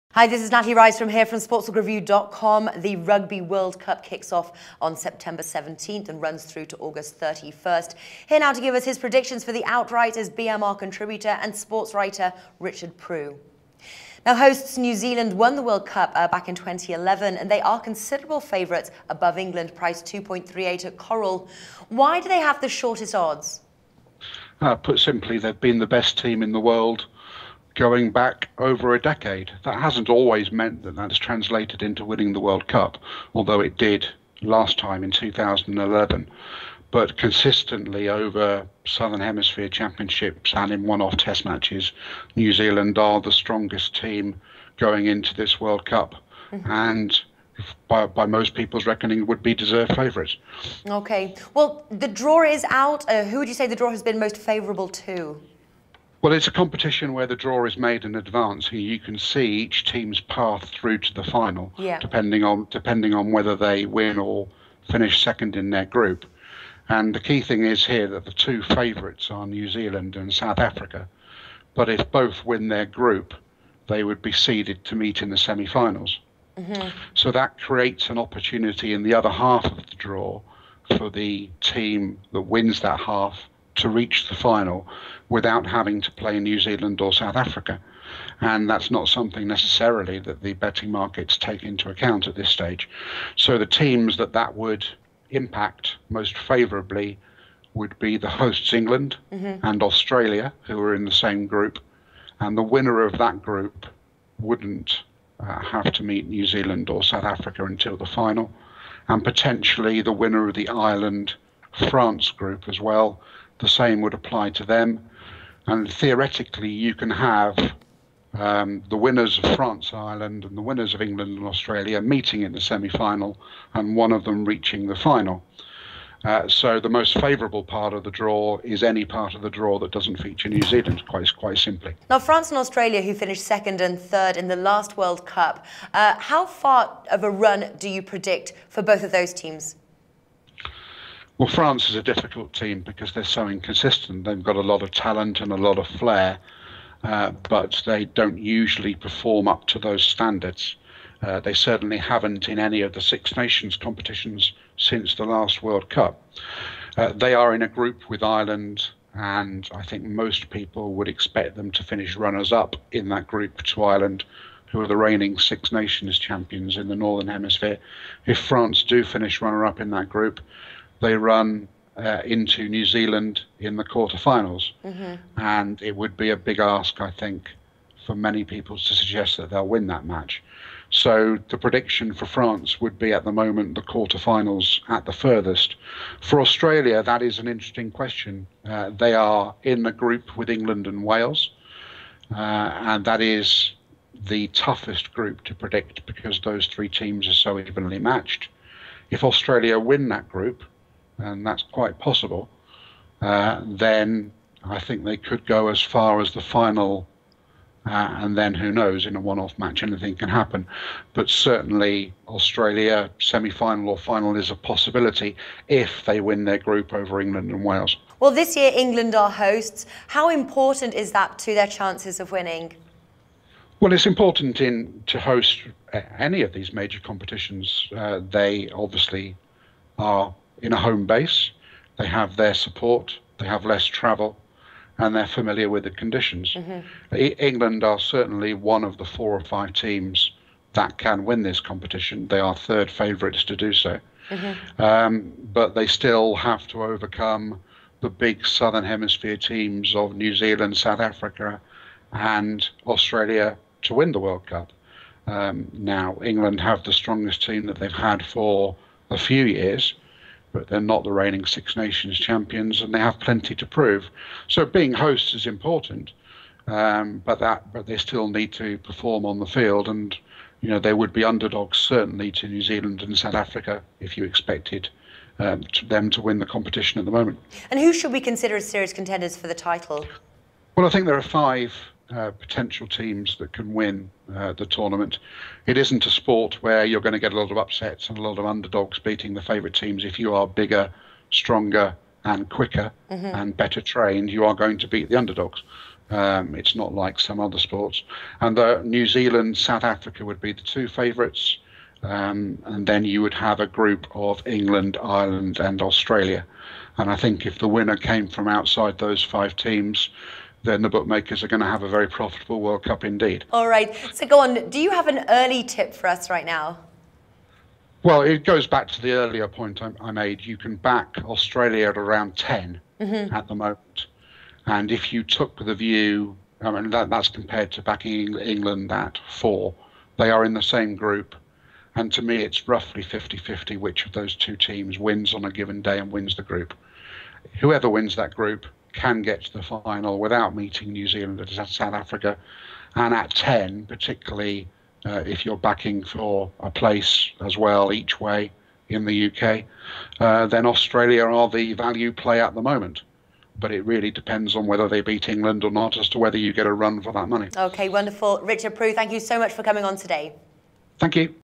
2015 Rugby World Cup - Outrights & Free Tips
The 2015 Rugby World Cup begins on September 17th and ends on October 31st. This time it will be hosted by England, giving them a slight advantage as the home team. New Zealand won the World Cup back in 2011 and this time they are also one of the favorites by oddsmakers to win the title, even above hosting team, England.  Sports presenter Natalie Rydstrom is joined by BMR contributor Richard Prew to discuss their predictions for the 2015 Rugby world Cup.